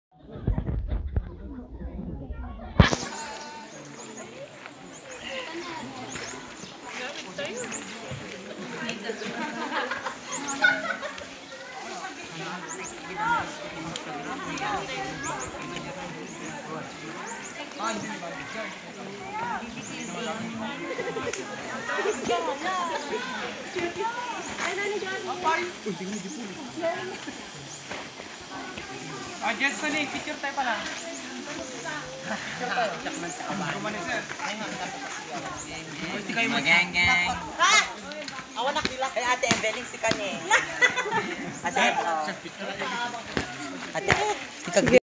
DUBA PART 4: CLEAN UP DRIVE
PART 4 ng CLEAN UP DRIVE AT DUBA SAN MIGUEL BAGGAO CAGAYAN

DJ THOR PHE TV ❤